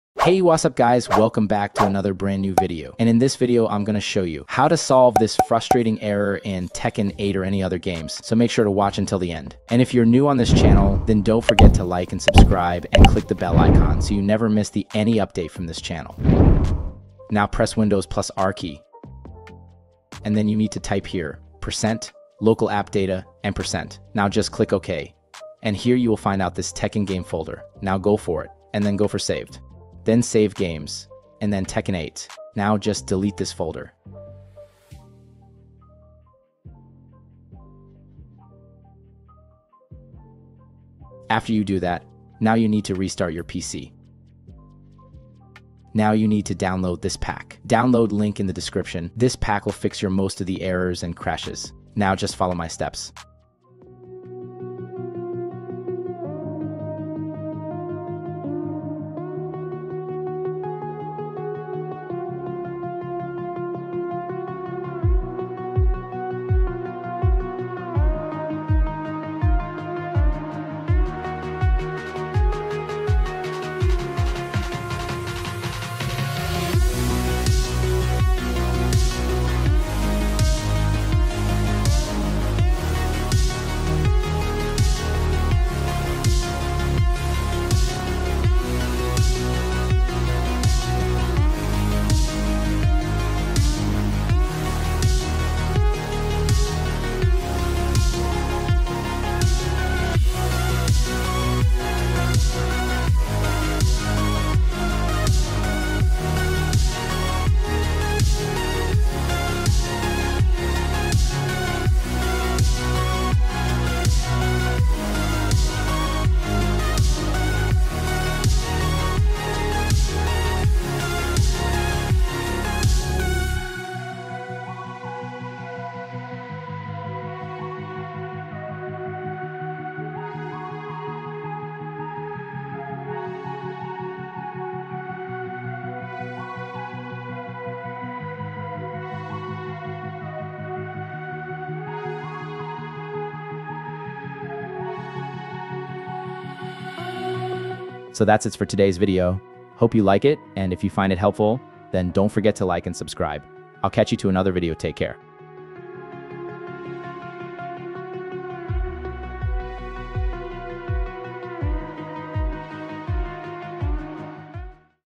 How To Fix Tekken 8 Crashes & Fatal Error | The UE-Polaris Game has crashed | Low Level Fatal Error
Title : How To Fix Tekken 8 Fatal Error & Crashes | The UE4-OMD Game has crashed Fix | Tekken 8 Crash Fixed

Videos Chapters :
00:00 intro
00:15 Deleting Files
00:38 Reboot Pc
00:46 Errors & Crashes Fix Pack
2:28 Ending

PC Spec's
Processor : Ryzen 5 5600
Motherboard : Msi B450m A-Pro Max
Gpu : XFX Rx 6600xt
Ram : Delta T-Force 16gb
PSU : Corsair TX750